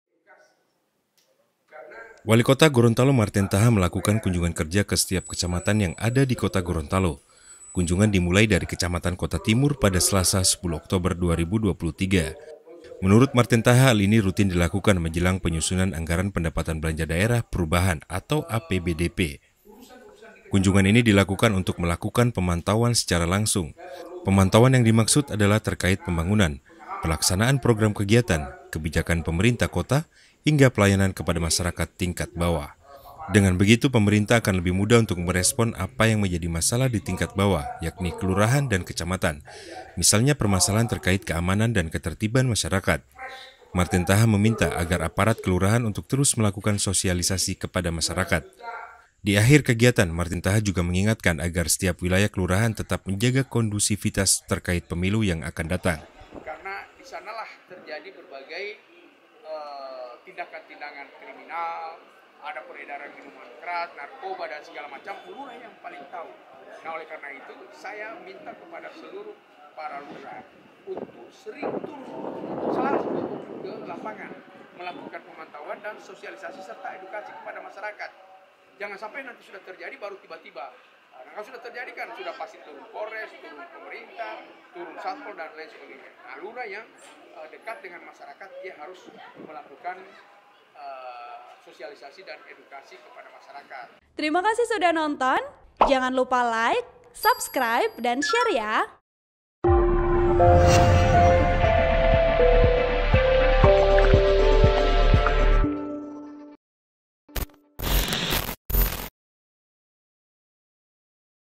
Jelang Penyusunan APBD-P, Wali Kota Gorontalo Marten Taha Turun Langsung ke Kecamatan
VP: Annisa Yulianti
TRIBUNGORONTALO.COM, Kota Gorontalo - Wali Kota Gorontalo Marten Taha melakuka kunjungan kerja ke setiap kecamatan yang ada di Kota Gorontalo. Kunjungan dimulai dari kecamatan Kota Timur pada Selasa (10/10/2023).

Artikel ini telah tayang di Tribungorontalo.com dengan judul Jelang Penyusunan APBD-P, Wali Kota Gorontalo Marten Taha Turun Langsung ke Kecamatan,
Penulis: Ahmad Rajiv Agung Panto | Editor: wawan akuba